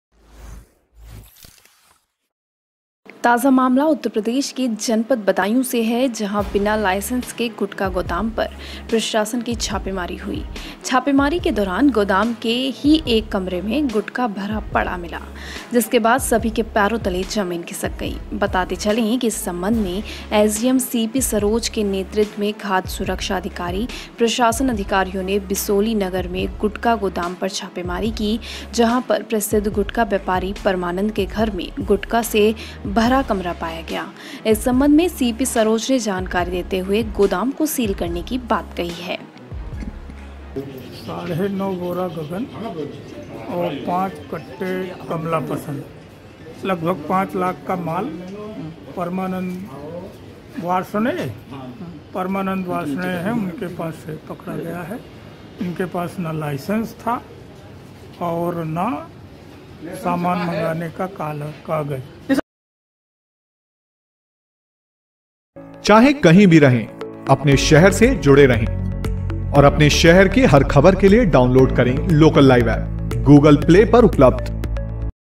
Badaun : बंद दुकान में मिला कई बोरी गुटखा, हुआ सील
ताजा मामला उत्तर प्रदेश के जनपद बदायूं से है जहाँ बिना लाइसेंस के गुटखा गोदाम पर प्रशासन की छापे मारी हुई। छापेमारी के दौरान गोदाम के ही एक कमरे में गुटखा भरा पड़ा मिला। जिसके बाद सभी के पैरों तले जमीन खिसक गई।
बताते चलें कि इस सम्बन्ध में एसडीएम सीपी सरोज के नेतृत्व में खाद्द सुरक्षा अधिकारी प्रशासन अधिकारियों ने बिसौली नगर में गुटका गोदाम पर छापेमारी की जहाँ पर प्रसिद्ध गुटखा व्यापारी परमानंद के घर में गुटका से भरा कमरा पाया गया। इस सम्बन्ध में सीपी सरोज ने जानकारी देते हुए गोदाम को सील करने की बात कही